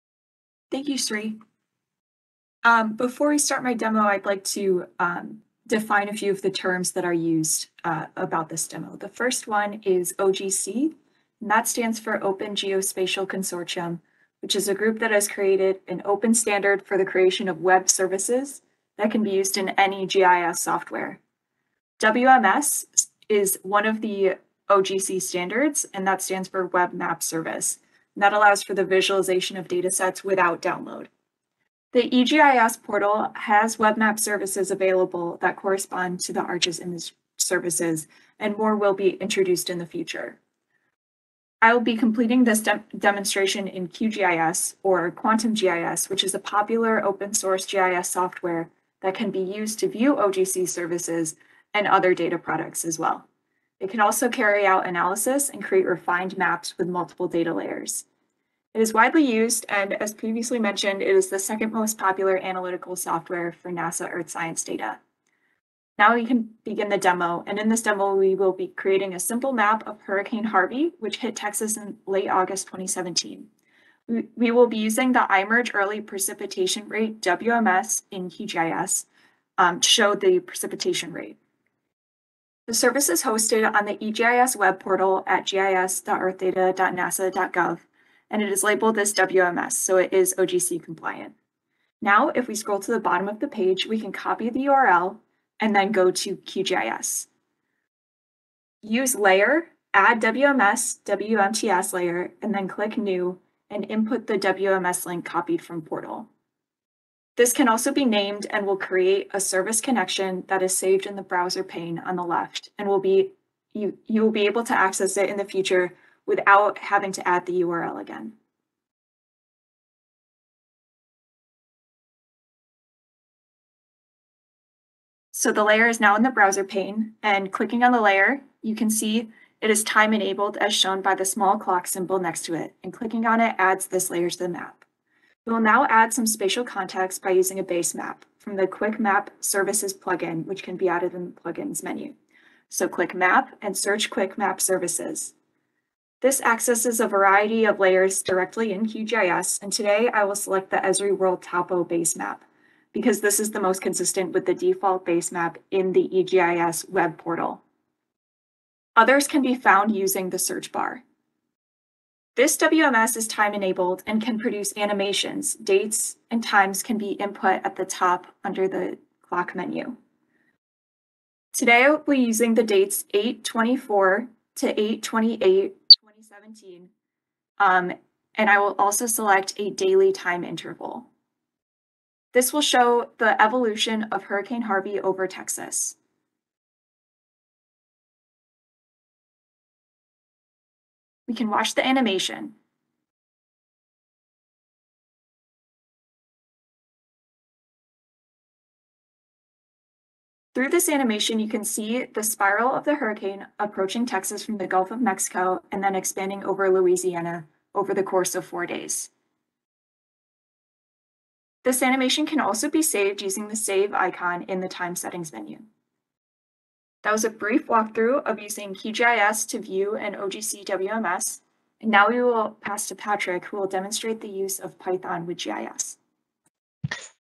GES DISC Demo: Leverage OGC WMS Endpoints in QGIS to Visualize Precipitation During Hurricane Harvey
Allison Alcott, of NASA's Goddard Earth Sciences Data and Information Services Center (GES DISC) demonstrates how to interact with the OGC WMS endpoints available through the Integrated Multi-satellitE Retrievals for GPM (IMERG) Early Precipitation Rate image service in QGIS. The WMS is time enabled and can be used to show the evolution of Hurricane Harvey over Texas.